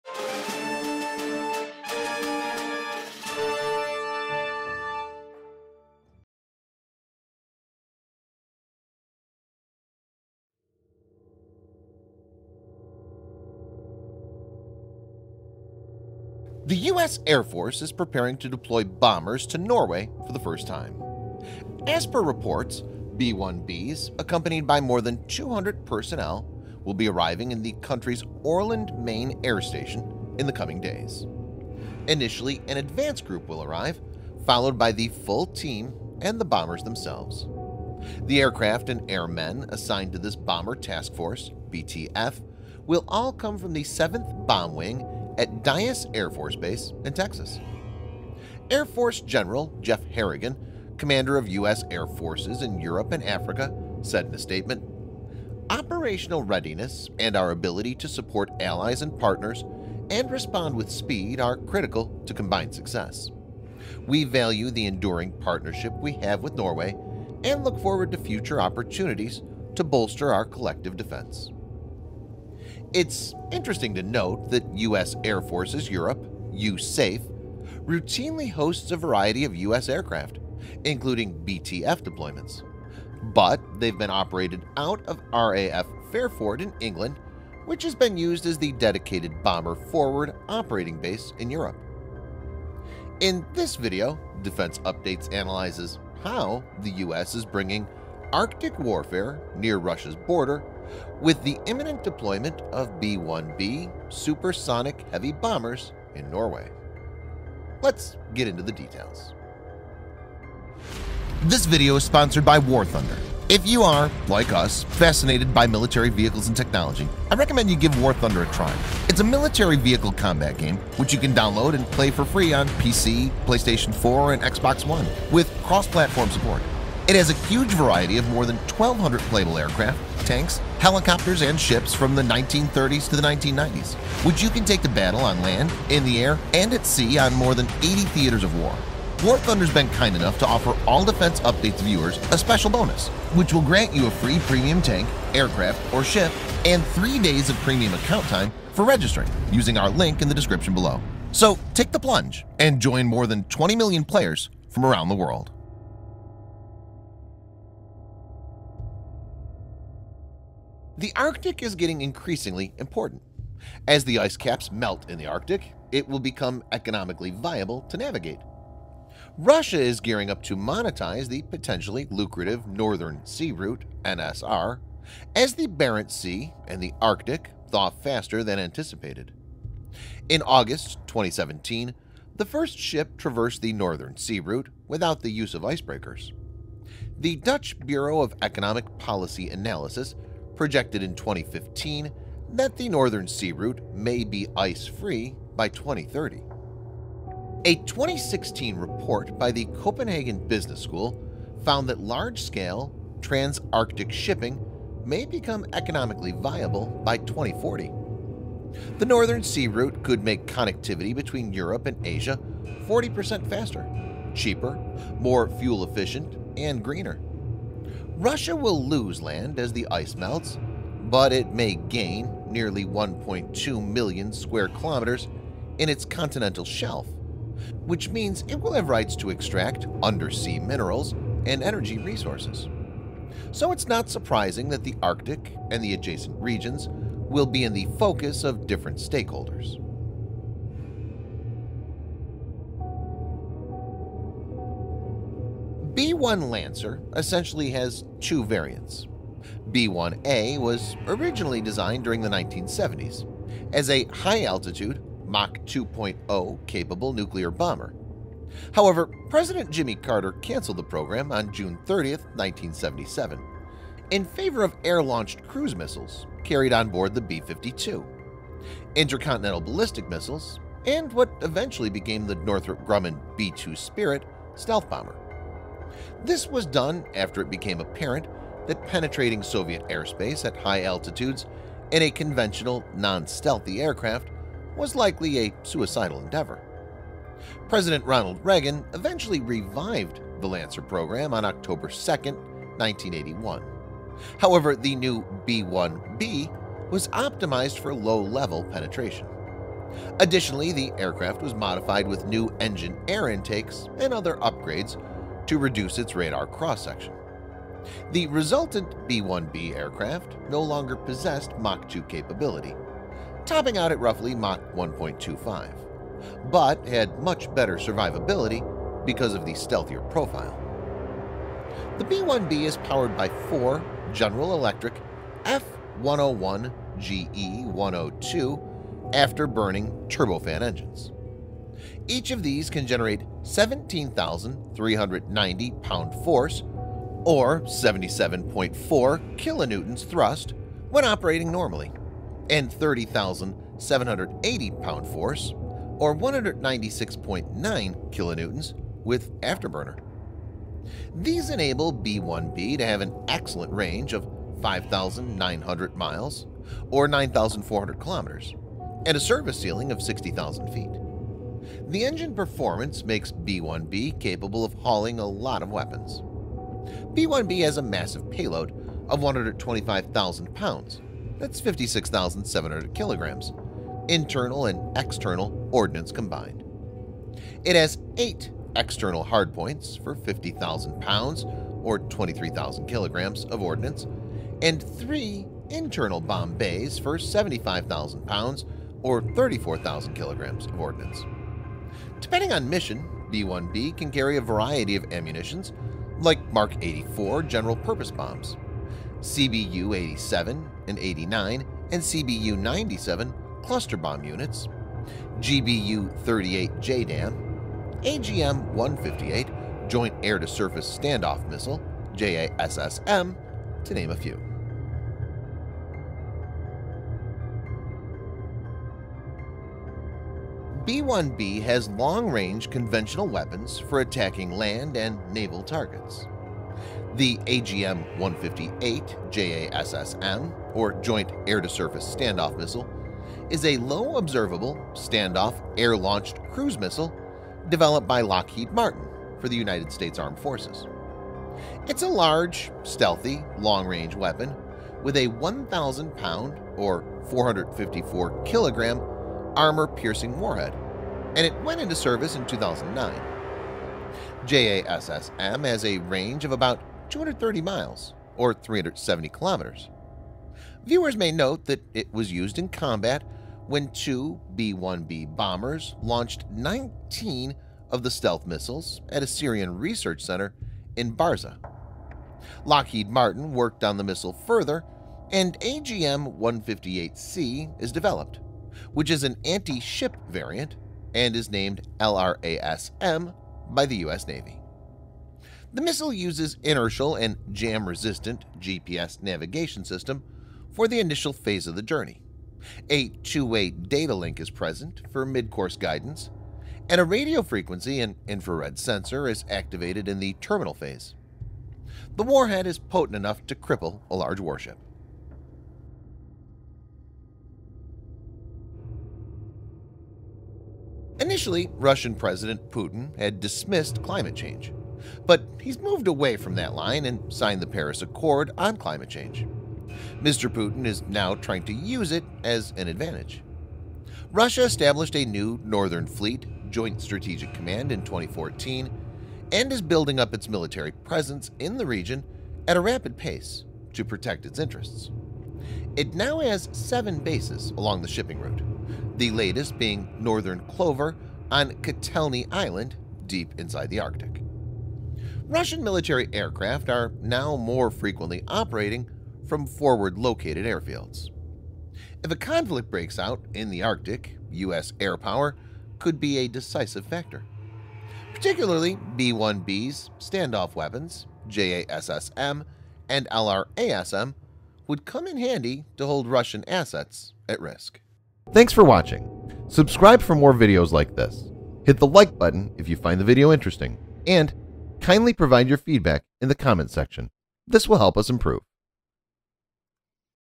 B-1B HEAVY BOMBERS WILL BE ARRIVING IN NORWAYS’s ØRLAND MAIN AIR STATION IN THE COMING DAYS !
The U.S. Air Force is preparing to deploy bombers to Norway for the first time.

As per reports, B-1Bs accompanied by more than 200 personnel will be arriving in the country’s Ørland Main Air Station in the coming days.
Initially, an advance group will arrive followed by the full team and the bombers themselves.

The aircraft and airmen assigned to this Bomber Task Force (BTF) will all come from the 7th Bomb Wing at Dyess Air Force Base in Texas.

Air Force General Jeff Harrigian, commander of U.S. Air Forces in Europe and Africa, said in a statement,
“Operational readiness and our ability to support allies and partners and respond with speed are critical to combined success. We value the enduring partnership we have with Norway and look forward to future opportunities to bolster our collective defense.”

It is interesting to note that U.S. Air Forces Europe (USAFE) routinely hosts a variety of U.S. aircraft, including BTF deployments, but they have been operated out of RAF Fairford in England, which has been used as the dedicated bomber forward operating base in Europe

In this video Defense Updates analyzes how the U.S is bringing Arctic Warfare Near Russia's Border with the imminent deployment of B-1B Supersonic Heavy Bombers in Norway?

Let's get into the details.

BACKGROUND MUSIC

IMAGES USES